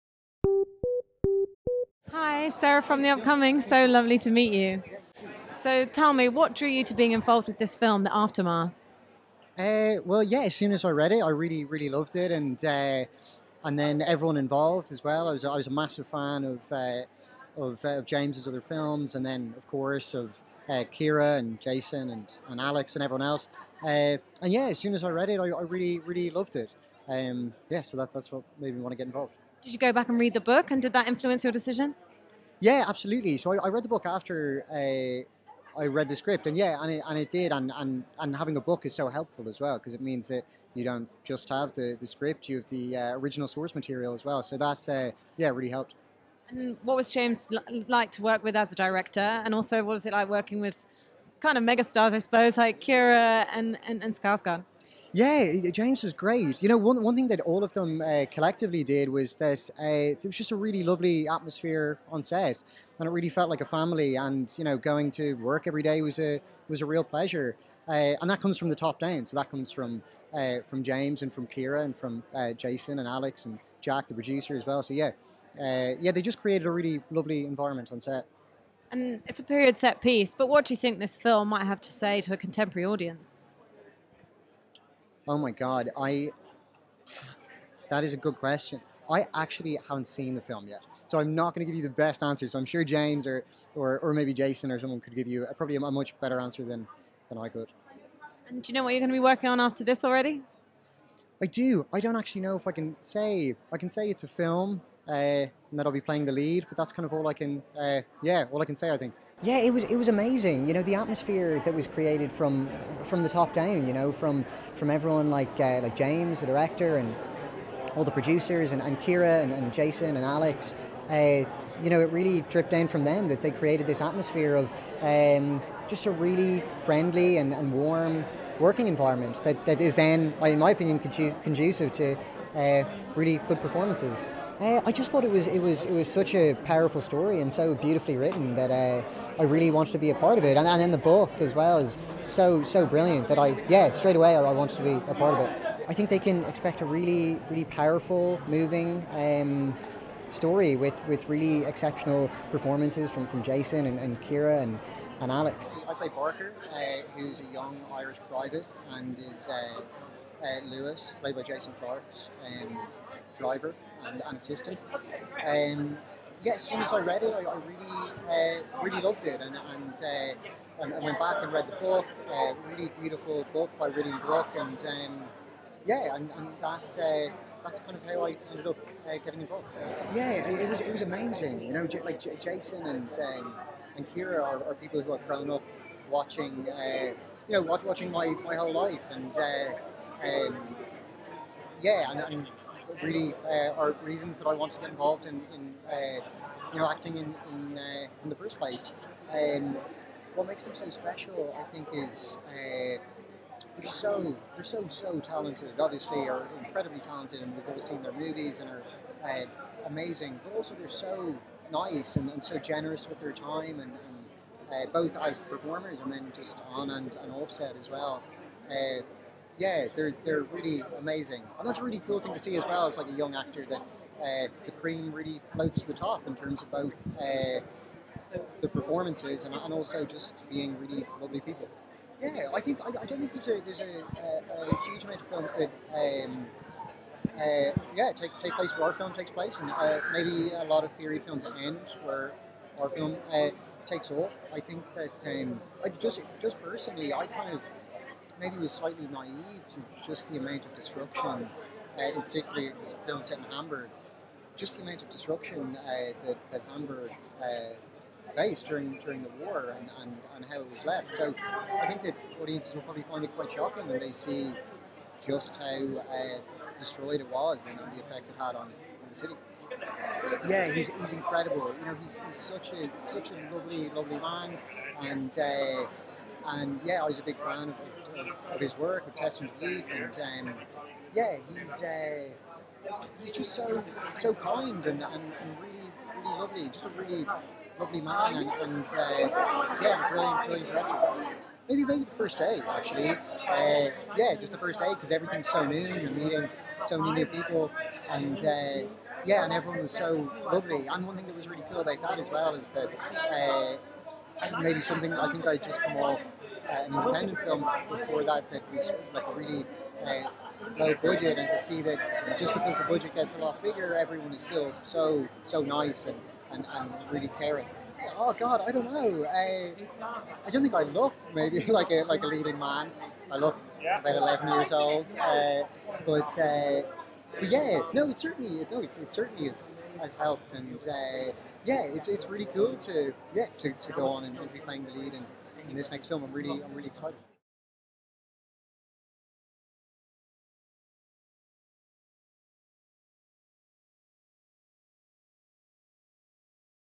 Fionn O'Shea on The Aftermath, Keira Knightley, Alexander Skarsgård at premiere
Interviewer: Sarah Bradbury (@sarabradbury)
Video: Marta Starczynowska
Editor: Filippo L'Astorina